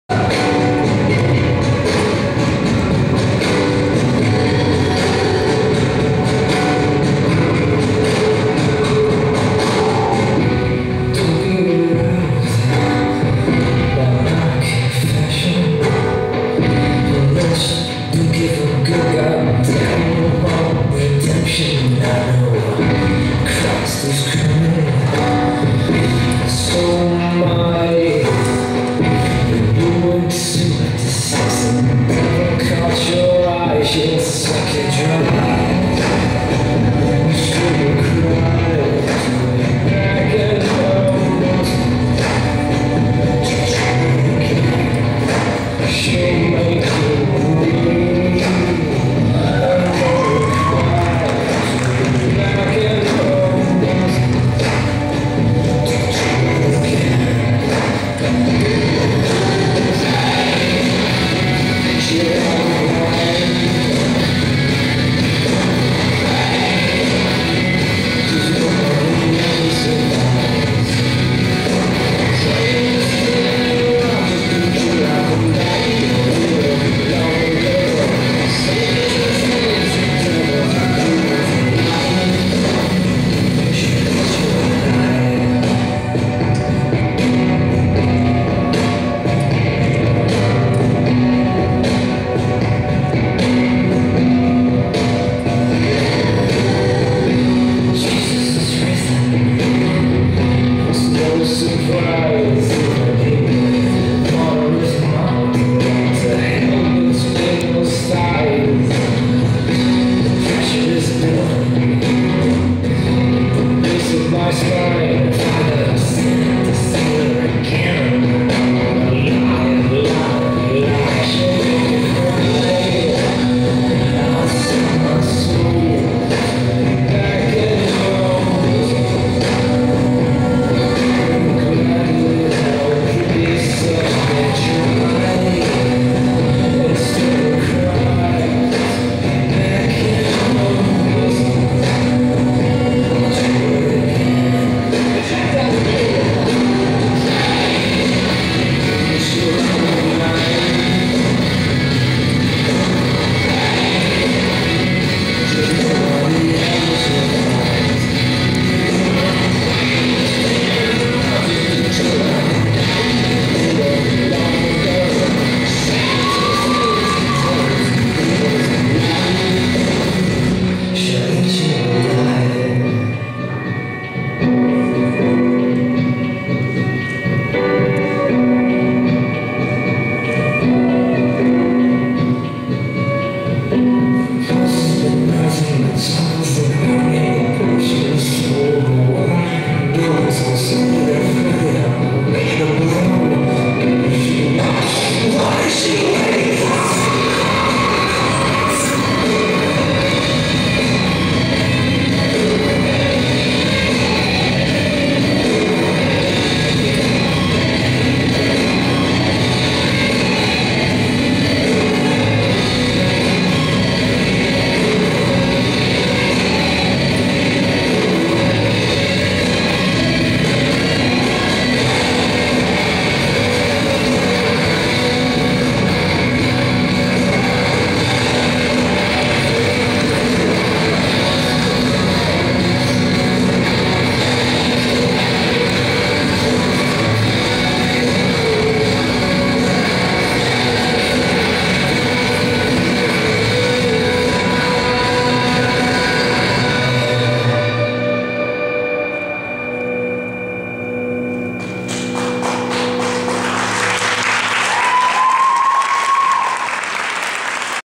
Asia- The Miss GA Pole Competition -pro division
Her 3rd round performance in The 2011 Miss Goergia Pole Dance Competition